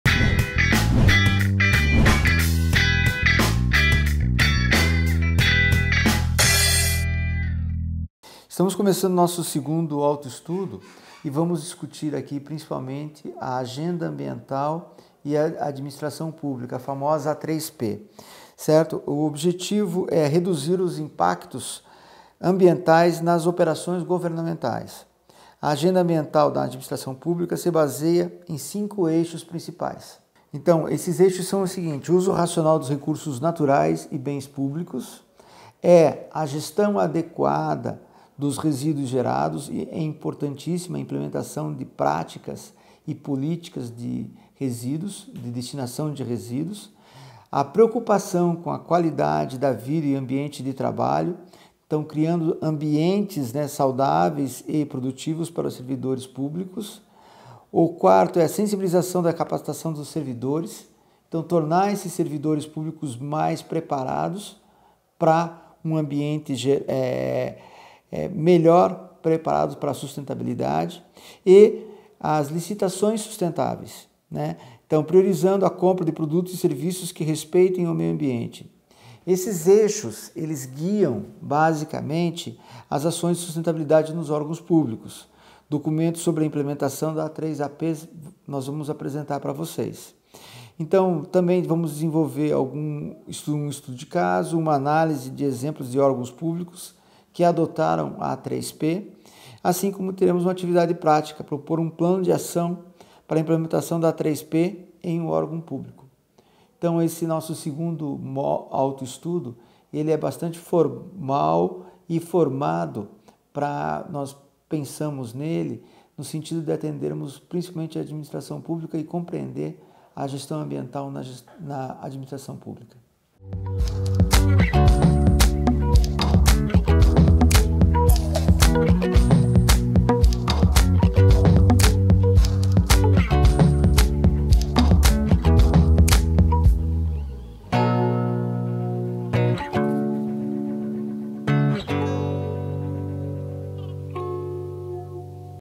[ADMP] - Agenda Ambiental e Administração Pública (A3P) - Auto estudo II
[ADMP] - Agenda Ambiental e Administração Pública (A3P) na disciplina de Gestão ambiental e Sustentabilidade com o professor Sergio Doliviera para o curso de Administração Pública - EaD da Universidade Estadual do Centro Oeste do Paraná.
Vídeo 3 - Auto estudo II